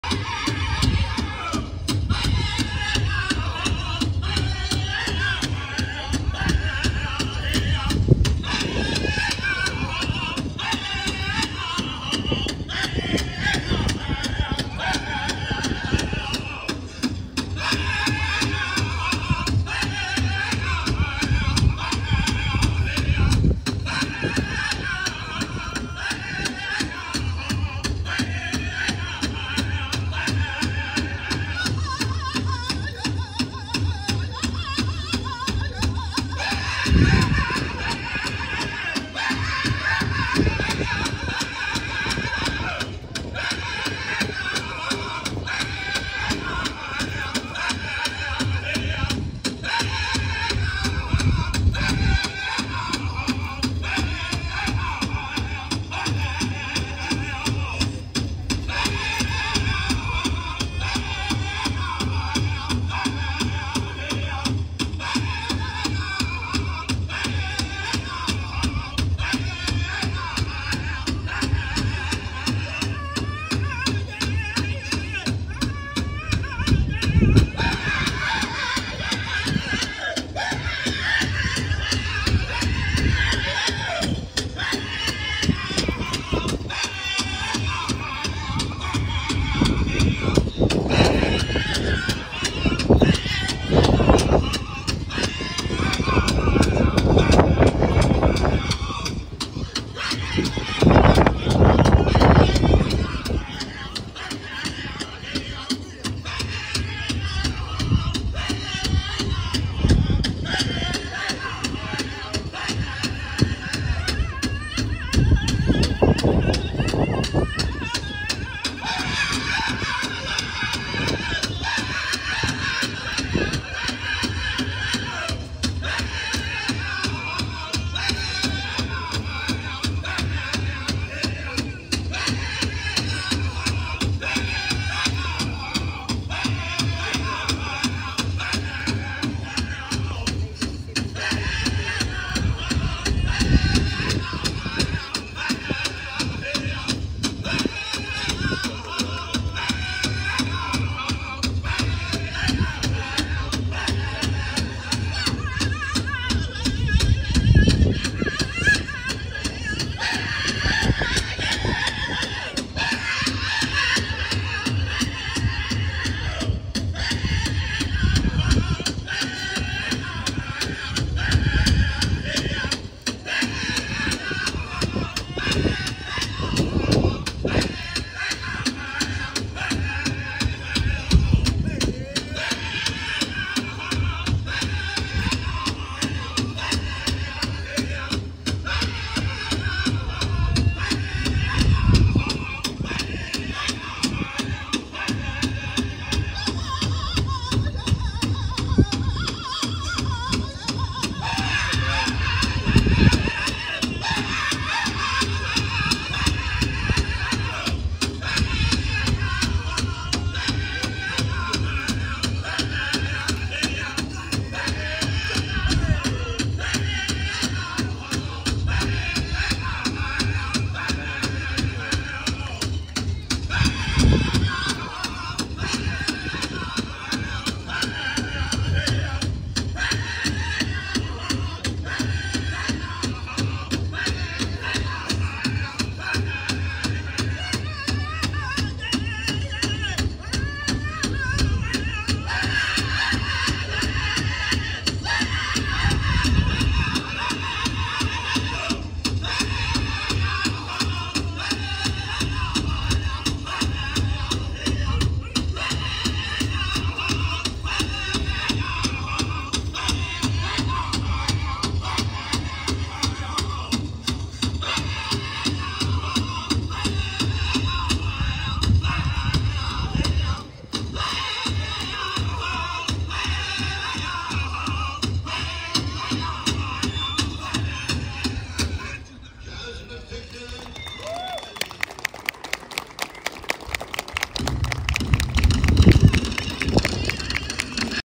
Lakota tribe hoop dance
Good dance performed at Mount Rushmore from I believe she said she was from the Lakota tribe in South Dakota black hills.